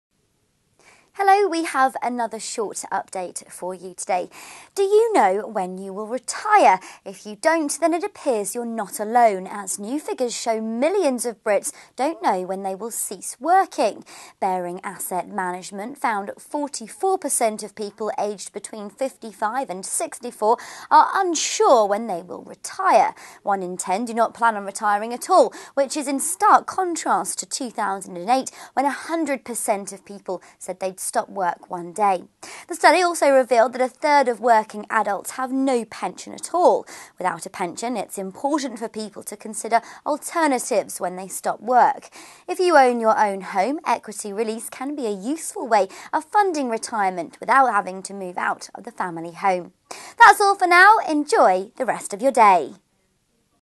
Do you know when you will retire?
Millions of Brits don't know when they will retire